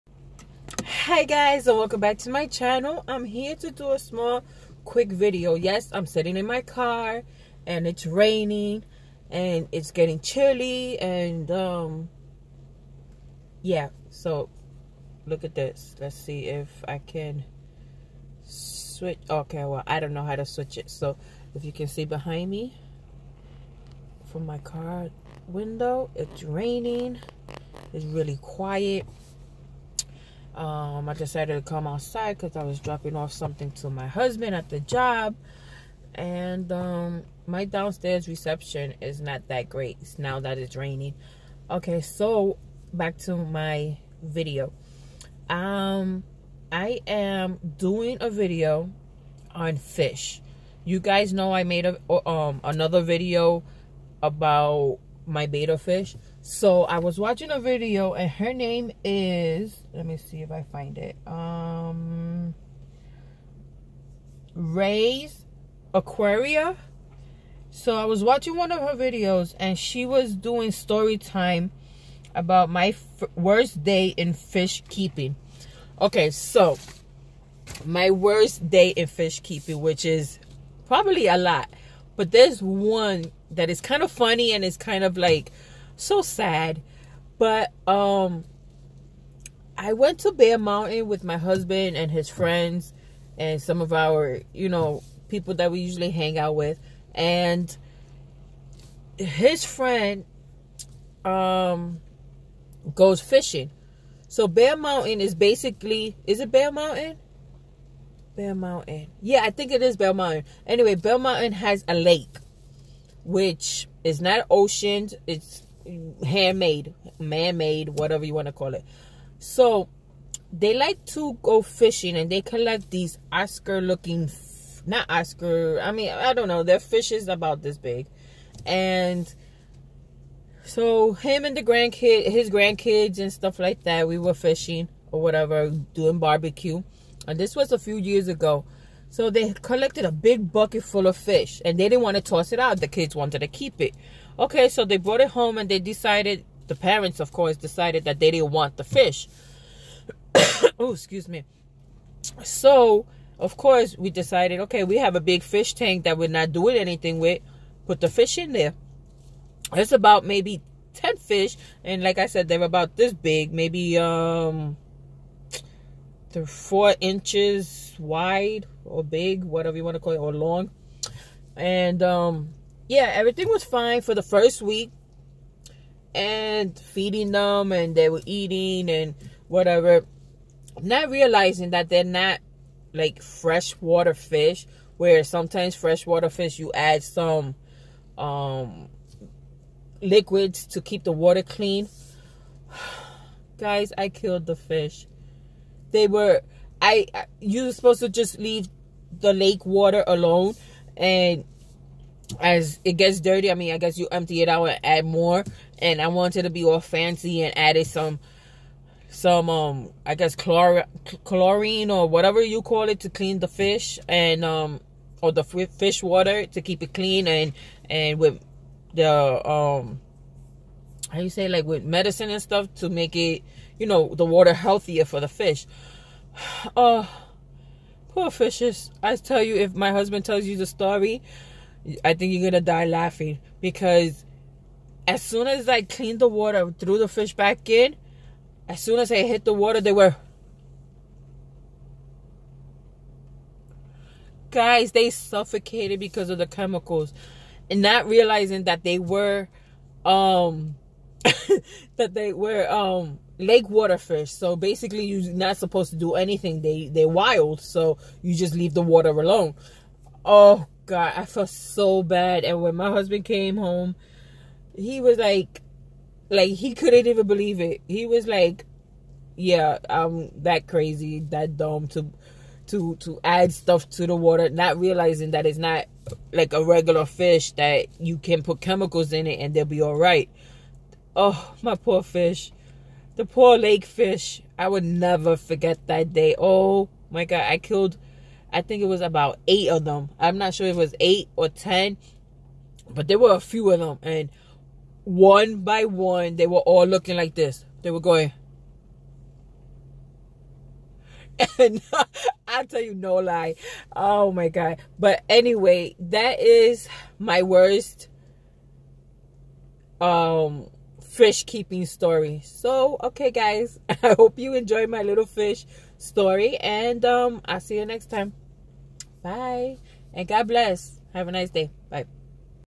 My worse day in fish keeping
I was watching a video by " rae's aquaria" and she did this kinda video.. so I figured I'll do the same since i also have betta fishes
Enjoy my story